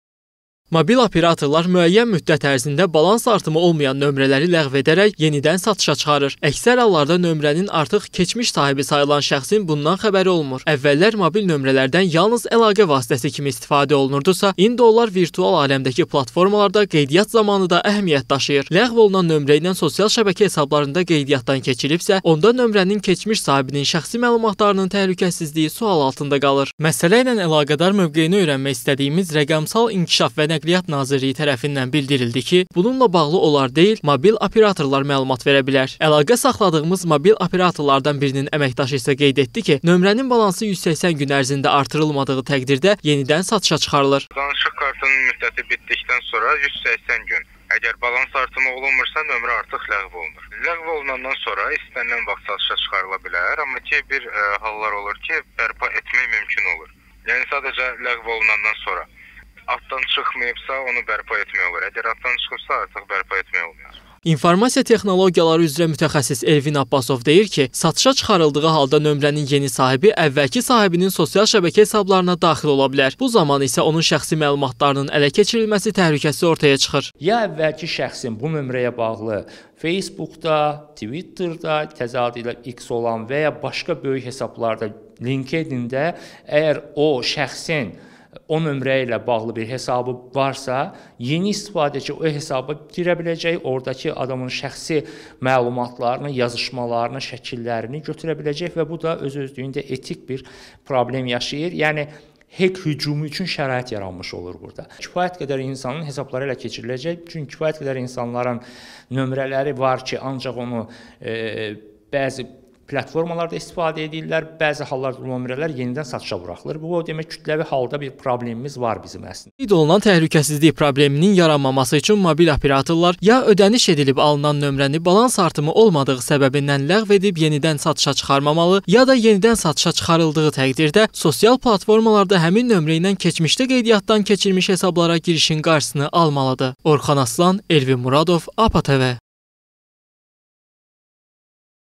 Mobil operatorlar nömrələrimizi başqasına satır, bütün məlumatlarınız ələ keçə bilər – APA TV
Mobil operatorlar müəyyən müddət ərzində balans artımı olmayan nömrələri ləğv edərək, yenidən satışa çıxarır. Əksər hallarda nömrənin artıq keçmiş sahibi sayılan şəxsin bundan xəbəri olmur. Əvvəllər mobil nömrələrdən yalnız əlaqə vasitəsi kimi istifadə olunurdusa, indi onlar virtual aləmdəki platformalarda qeydiyyat zamanı da əhəmiyyət daşıyır. Ləğv olunan nömrə ilə sosial şəbəkə hesablarında qeydiyyatdan keçilibsə, onda nömrənin keçmiş sahibinin şəxsi məlumatlarının təhlükəsizliyi sual altında qalır.
Mövzu ilə əlaqədar mövqeyini öyrənmək istədiyimiz Rəqəmsal İnkişaf və Nəqliyyat Nazirliyi tərəfindən bildirildi ki, bununla bağlı onlar deyil, mobil operatorlar məlumat verə bilər. Əlaqə saxladığımız mobil operatorlardan birinin əməkdaşı isə qeyd etdi ki, nömrənin balansı 180 gün ərzində artırılmadığı təqdirdə yenidən satışa çıxarılır.
İnformasiya Texnologiyaları üzrə mütəxəssis Elvin Abbasov deyir ki, yenidən satışa çıxarıldığı halda nömrənin yeni sahibi əvvəlki sahibinin sosial şəbəkə hesablarına daxil ola bilər. Bu zaman isə onun şəxsi məlumatlarının ələ keçirilməsi təhlükəsi ortaya çıxır.
Qeyd olunan təhlükəsizlik probleminin yaranmaması üçün mobil operatorlar ya ödəniş edilib alınan nömrəni balans artımı olmadığı səbəbindən ləğv edib yenidən satışa çıxarmamalı, ya da yenidən satışa çıxarıldığı təqdirdə sosial platformalarda həmin nömrə ilə keçmişdə qeydiyyatdan keçilmiş hesablara girişin qarşısını almalıdır.
ORXAN ASLAN
ELVİN MURADOV
APA TV